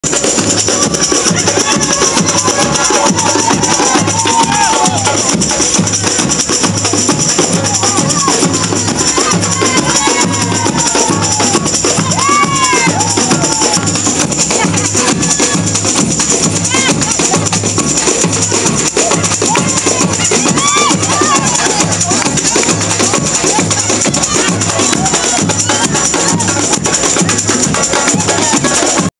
Schiaffo sul sedere di una ballerina di samba alla maratona di capodanno 2013 a Roma
Ballerina di samba molestata da maratoneta in corsa:
Schiaffo sul sedere di una ragazza alla maratona di capodanno 2013 a Roma

Dec.31 - 2013 - New Year's Eve marathon in Rome. Marize De Souza is the name of the brazilian dancer.